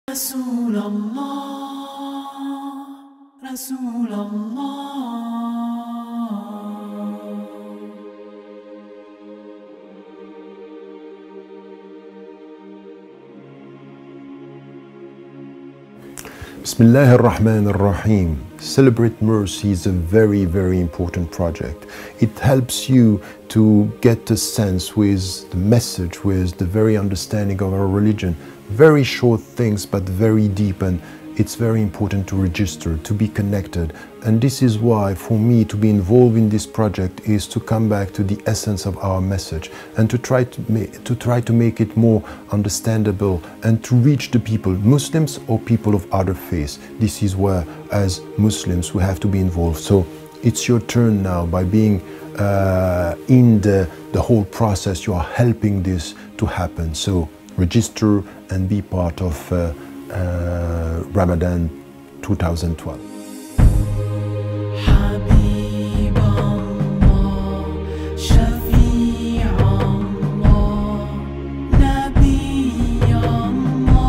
Dr. Tariq Ramadan promotes "The Dawn of Mercy: Muhammad in Mecca" CelebrateMercy Webcast
Register today for the global webcast "The Dawn of Mercy: Muhammad in Mecca." At CelebrateMercy.com you can register yourself or your group, request free flyers to be mailed to you, and sign up as a volunteer. Each year, thousands of viewers from over 90 countries participate in our live interactive webcasts featuring 25 of the most prominent Muslim scholars, speakers, and performers.